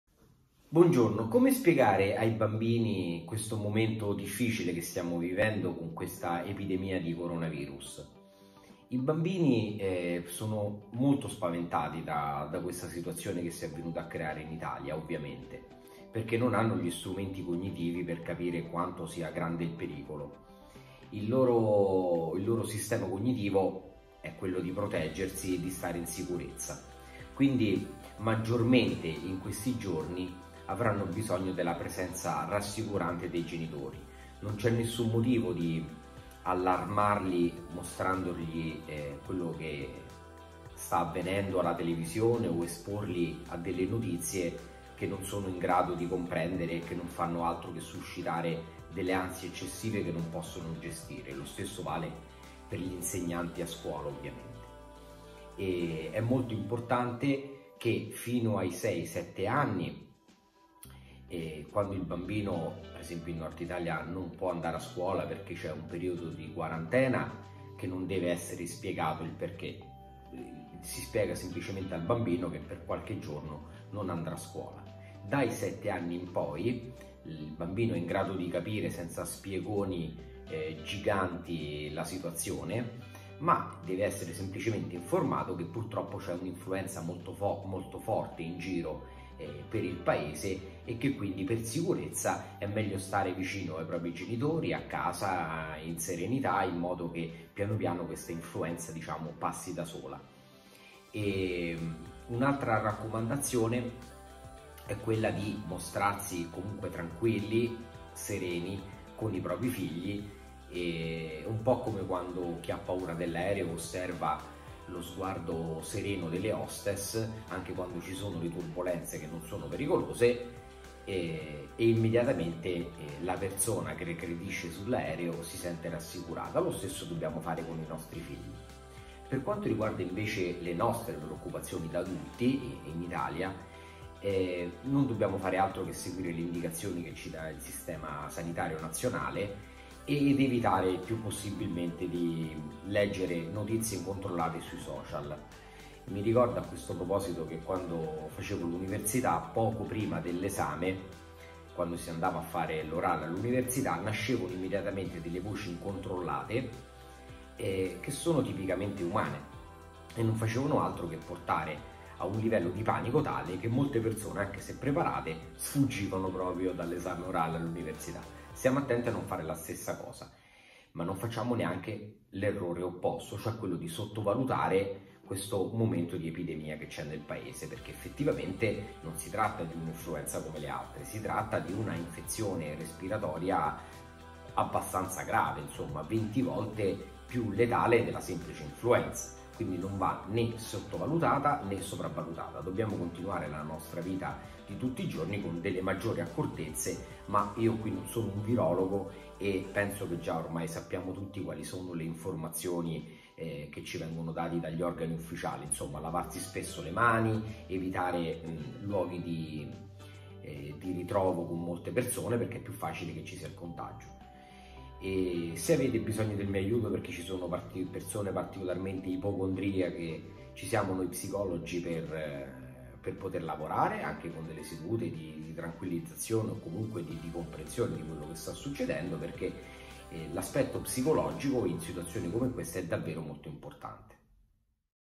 Come spiegare ai bambini il Coronavirus?
Il dott. Stefano Scatena ci parla di come non traumatizzare i nostri figli per l’epidemia di Covid-19 in corso in Italia